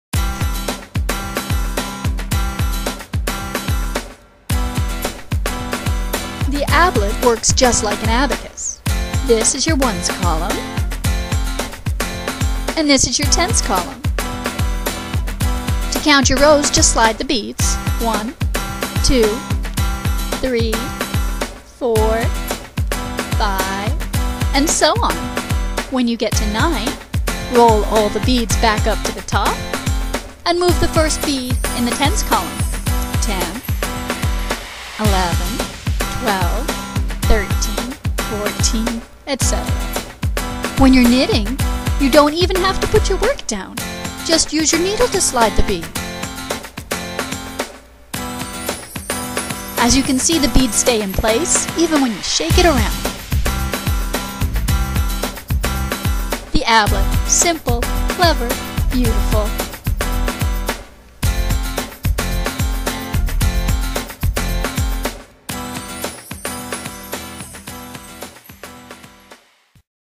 The ABLET knitting abacus bracelet!
This patent pending item is a functional, fashionable piece of jewelry for all knitters and crocheters. The colorful beads easily slide to count rows or patterns while staying in securely in place. It is hand made with a fully adjustable, silver-plated toggle clasp. It makes a perfect gift or self purchase item!